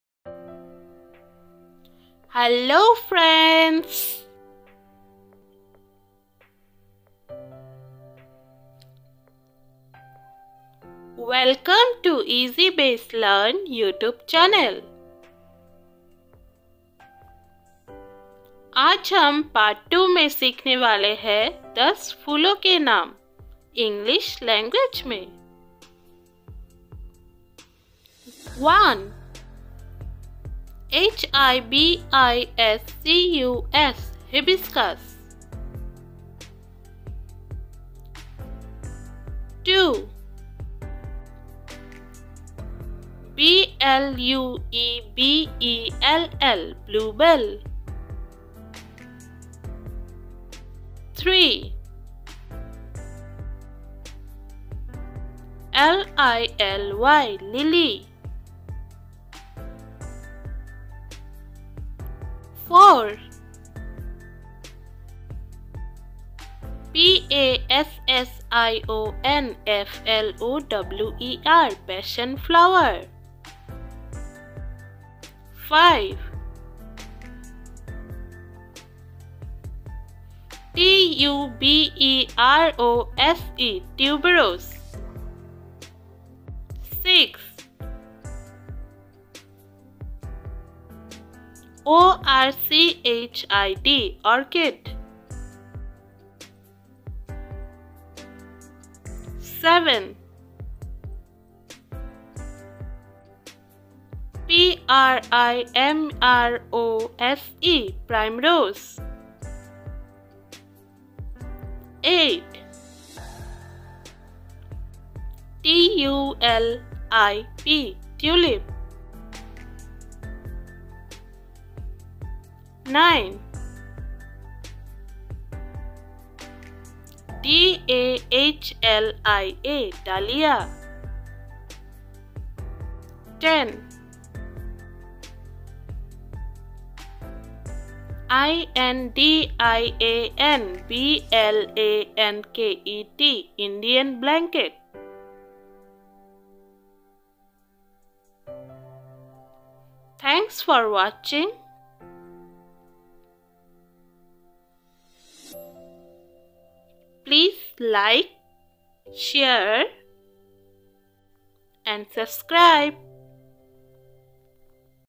Flowers Name |Learn Flowers Name In English | Flowers Name With Pictures And Spellings | 10 Flowers
Hello Friends , Welcome To Easy Base Learn YouTube Channel. In This Video You Can See Pre School Education, Nursery Videos, Flowers Name In English  (Part 02). You Can Easily Understand Or  Learn Flowers Name.